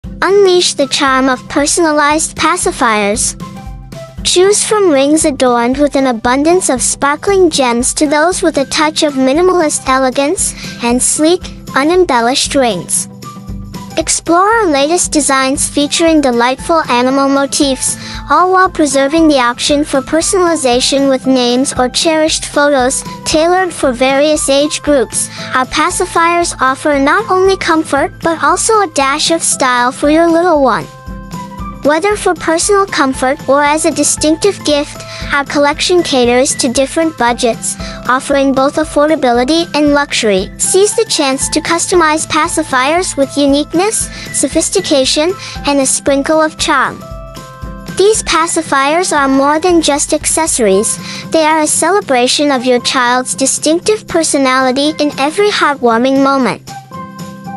Personalized Pacifiers: A Touch of Love for Your Little One!
Looking for the perfect pacifier for your little one? Look no further! Our personalized pacifiers offer a range of colors, styles, and the option to add your baby's name. From classic to unique designs, these pacifiers are not only practical but also add a touch of love to every moment.